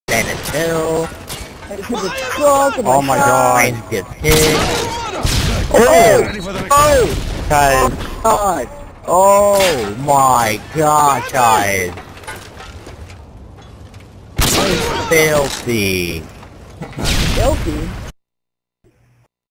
Chives @ Scrapyard!!!
Nice killcam I just hit on Scrapyard. SUB (: